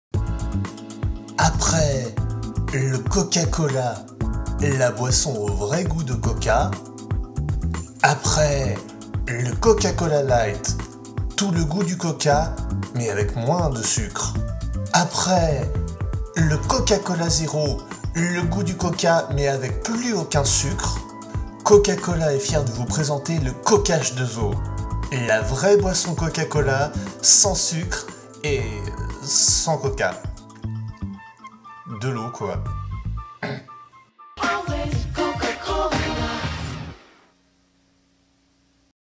Le Coca H2O
A force de mettre de moins en moins de sucre dans le Coca, voici ce qui a arrive...